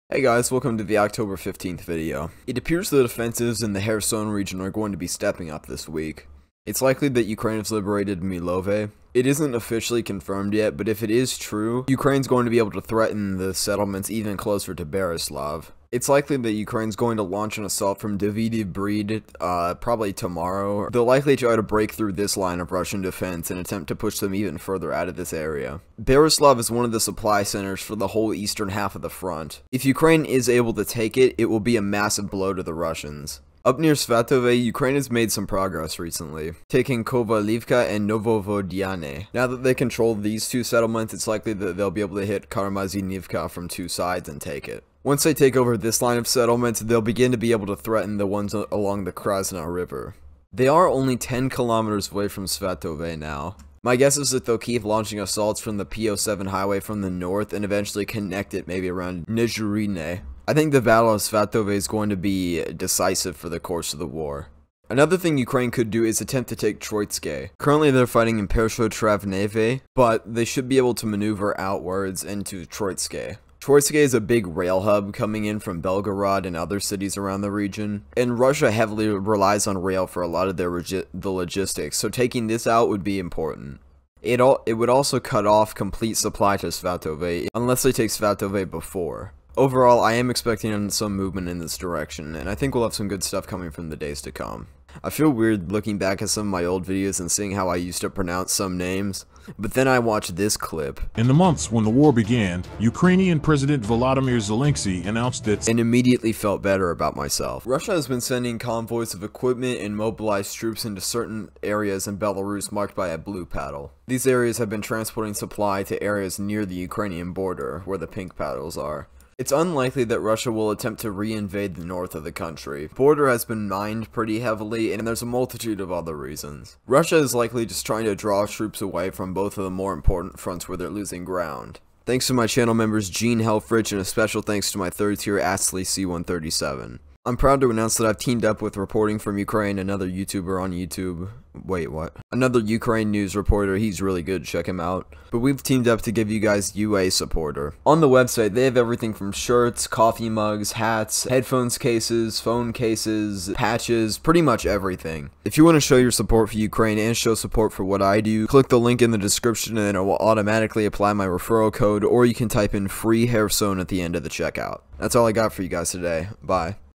Kherson Front Heating Up! | October 15th
Covering even more of the pushes Ukraine made around Kherson